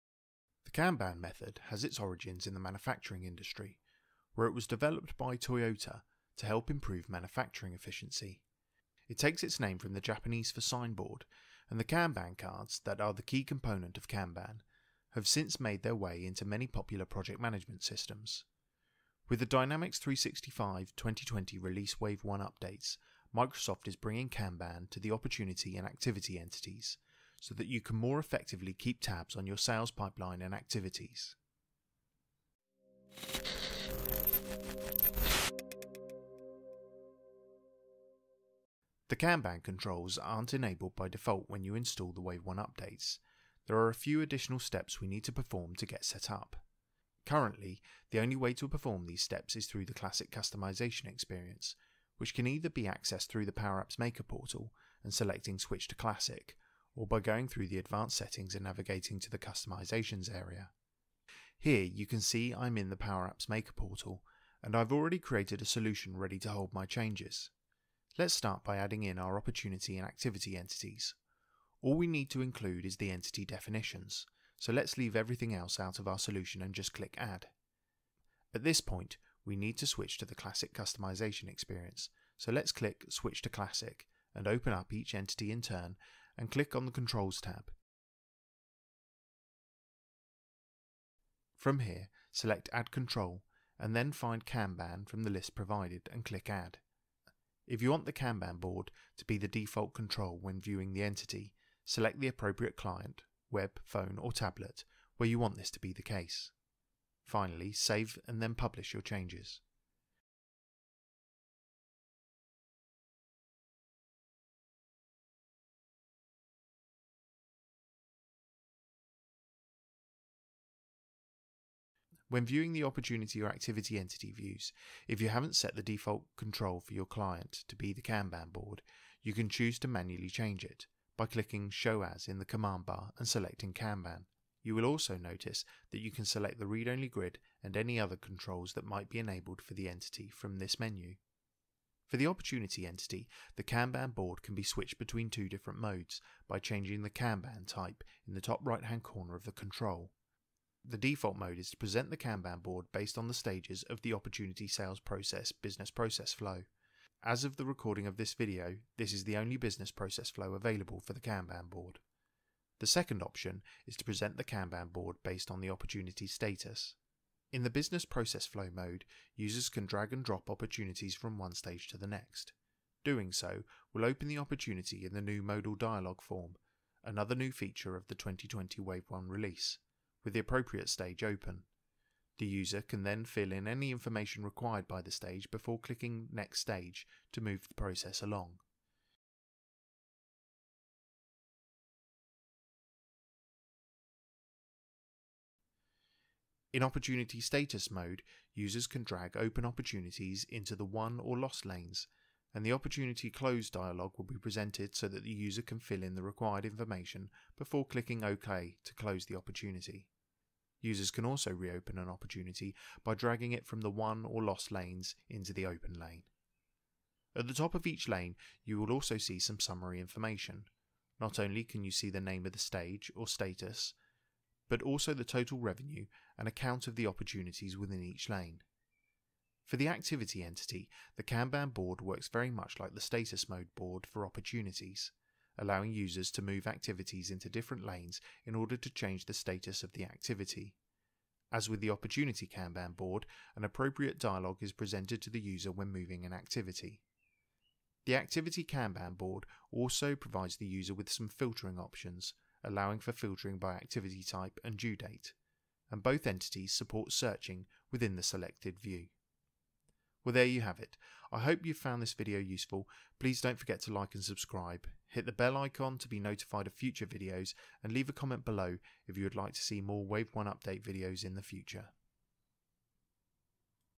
Enable the Kanban Board for Dynamics 365 to more easily manage your opportunities and activities
Find out how to enable the new Kanban Board for Dynamics 365 in order to take control of your opportunities and activities in the Dynamics 365 2020 release wave 1 update.